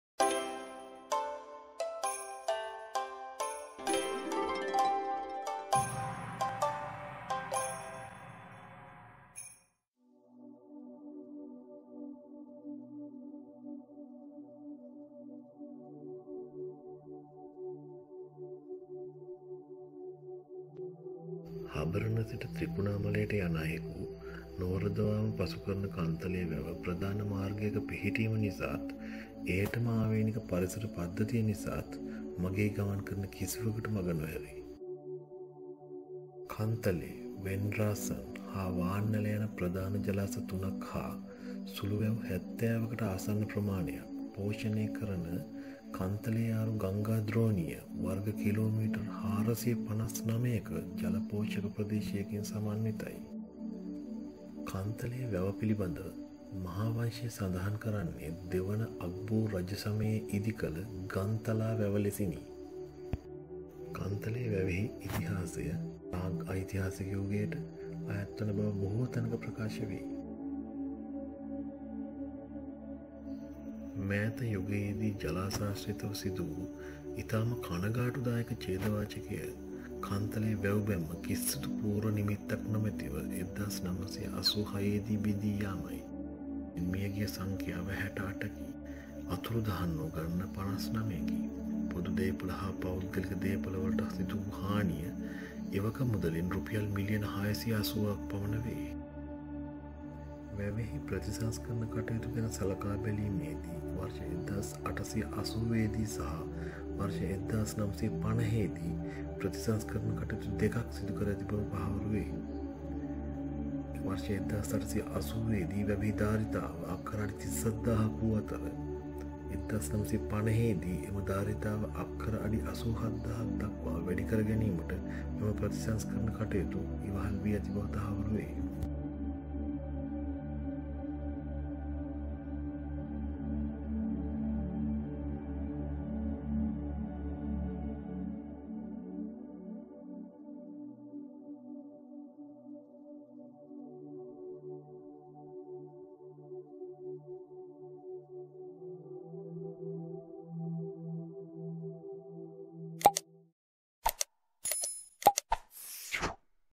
කන්තලේ වැව
විවිධ වකවානු වල ශ්‍රී ලංකාවේ සංචාරය කොට සටහන් ලියා තැබූ අය අතින් මහත් වර්ණනාවට ලක් වූ පැරණි ජලාශය කුමක් ද යන්න විමසුව හොත් මුල් ම තැන හිමි වන්නේ කන්තලේ වැවට ය. හබරණ සිට ත්‍රිකුණාමලයට යන අයෙකු නොවරදවාම පසු කරන කන්තලේ වැව, ප්‍රධාන මාර්ගයක පිහිටීම නිසාත් එයට ම ආවේණික පරිසර පද්ධතිය නිසාත් මෙම මගෙහි ගමන් කරන කිසිවකුට මඟ නොහැරේ. කන්තලේ වැව පිහිටි කන්තලේ ආරු, අනුරාධපුර දිස්ත්‍රික්කයේ හුරුළු වැව රක්ෂිතයේ නැගෙනහිර බෑවුමෙන් ගමන් ආරම්භ කර, මහවැලි මෝයට උතුරින් මුහුදට එකතු වන්නේ මිනිසාට, සතා සීපාවට මෙන් ම පරිසරයට ද මහත් සේවයක් සිදු කරමිනි

 කන්තලේ, වෙන්ඩ්‍රාසන් හා “වාන්ඇල” යන ප්‍රධාන ජලාශ 3 ක් හා සුළු වැව් 70 කට ආසන්න ප්‍රමාණයක් පෝෂණය කරන කන්තලේ ආරු ගංගා ද්‍රෝණිය වර්ග කි. මී. 459 ක ජල පෝෂක ප්‍රදේශයකින් සමන්විතය

 කන්තලේ වැව පිළිබඳ මහාවංශය සඳහන් කරන්නේ දෙවන අග්බෝ රජසමයේ ඉදි කළ ගංතලා වැව ලෙසිනි.